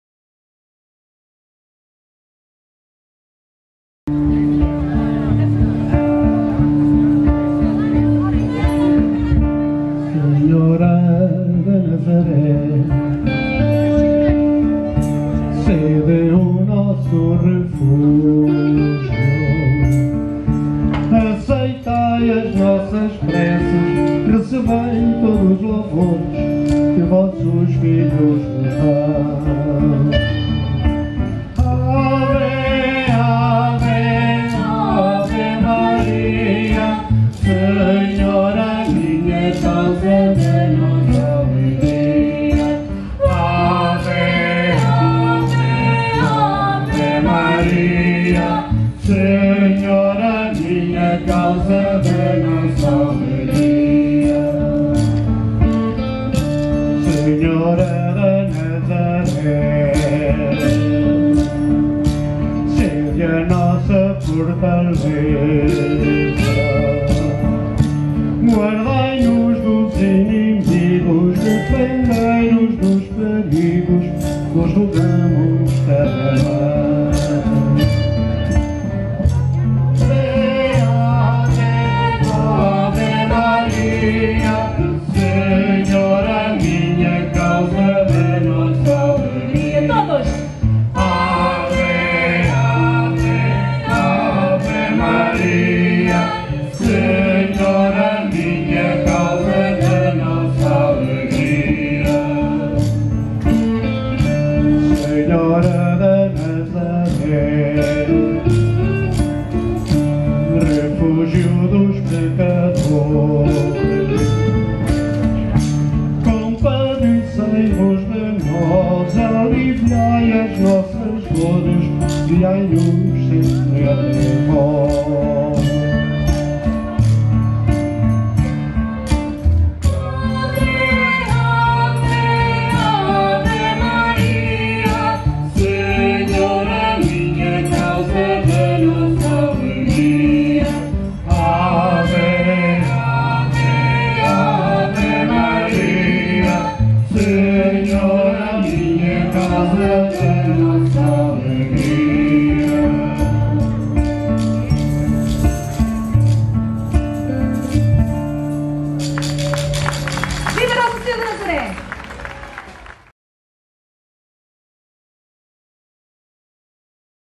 Cavaquinhos do Oeste [Senhora da Nazaré] Terrugem Sintra
Actuação nas Festas da Senhora da Nazaré em Terrugem-Sintra, no dia 21SET2019